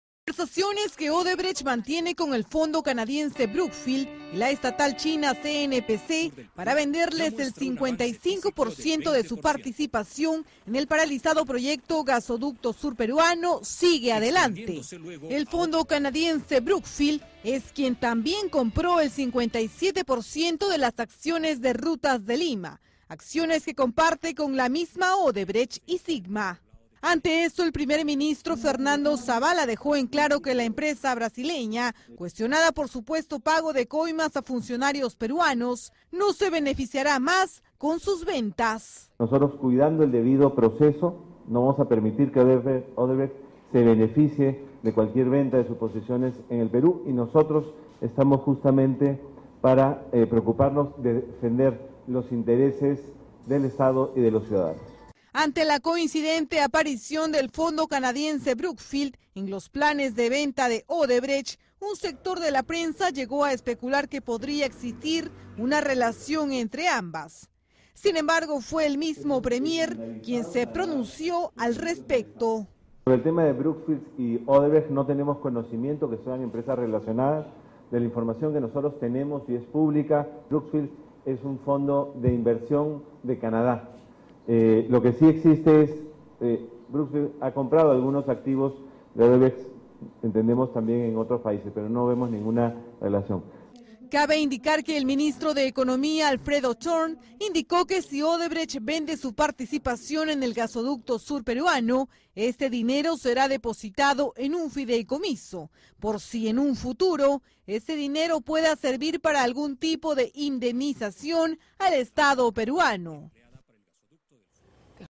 Gobierno impedirá que Odebrecht se beneficie con venta de sus activos en Perú
El primer ministro Fernando Zavala dejó en claro que la empresa brasileña Odebrecht, cuestionada por supuesto pago de coimas a funcionarios peruanos, no se beneficiará más con sus ventas.